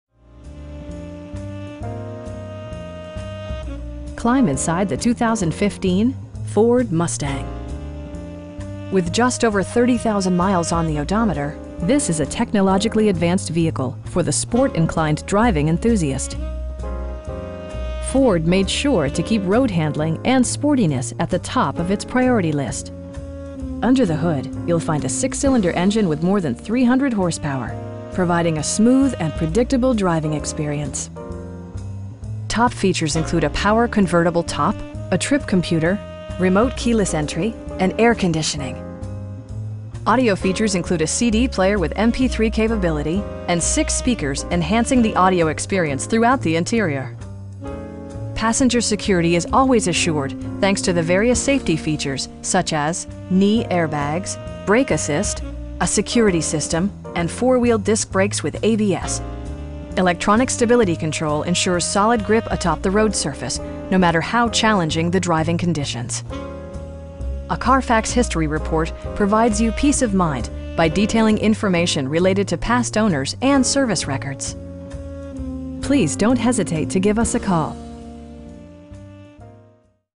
2015 Ford Mustang V6 in San Diego, CA 92111
Volkswagen of Kearny Mesa
8040 Balboa Avenue

Climb inside the 2015 Ford Mustang. With just over 30,000 miles on the odometer, this is a technologically advanced vehicle for the sport-inclined driving enthusiast! Ford made sure to keep road-handling and sportiness at the top of it's priority list. Under the hood you'll find a 6 cylinder engine with more than 300 horsepower, providing a smooth and predictable driving experience. Top features include a power convertible top, 1-touch window functionality, a trip computer, front bucket seats, power windows, remote keyless entry, cruise control, and air conditioning. With high intensity discharge headlights illuminating your path, you'll always appreciate maximum visibility. Audio features include a CD player with MP3 capability, steering wheel mounted audio controls, and 6 speakers, enhancing the audio experience throughout the interior. Passenger security is always assured thanks to various safety features, such as: dual front impact airbags, knee airbags, traction control, brake assist, a security system, and 4 wheel disc brakes with ABS. Electronic stability control ensures solid grip atop the road surface, no matter how challenging the driving conditions. A Carfax history report provides you peace of mind by detailing information related to past owners and service records. Our aim is to provide our customers with the best prices and service at all times. Please don't hesitate to give us a call.